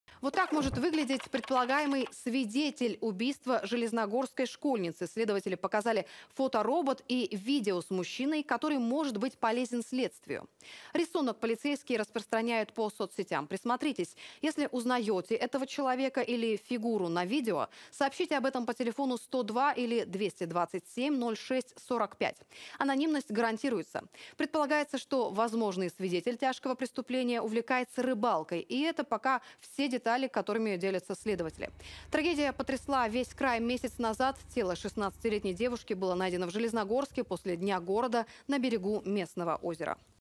Следователи ищут возможного свидетеля убийства школьницы
Вот так может выглядеть предполагаемый свидетель убийства железногорской школьницы. Следователи показали фоторобот и видео с мужчиной, который может быть полезен следствию. Рисунок мужчины полицейские распространяют по соц.сетям. Присмотритесь, если узнаёте этого человека или фигуру на видео, сообщите об этом по телефону 102 или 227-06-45. Анонимность гарантируется. Предполагается, что возможный свидетель тяжкого преступления увлекается рыбалкой. Это пока все детали, которыми делятся следователи. Напомним, труп 16-летней девушки был найден в Железногорске после дня города на берегу местного озера почти месяц назад.